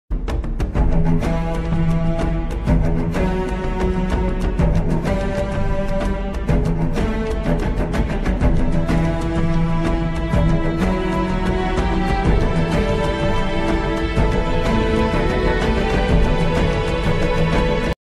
Halo Mega construx stop motion: Covenant weapon test
once again I don't have the sound effects for the weapons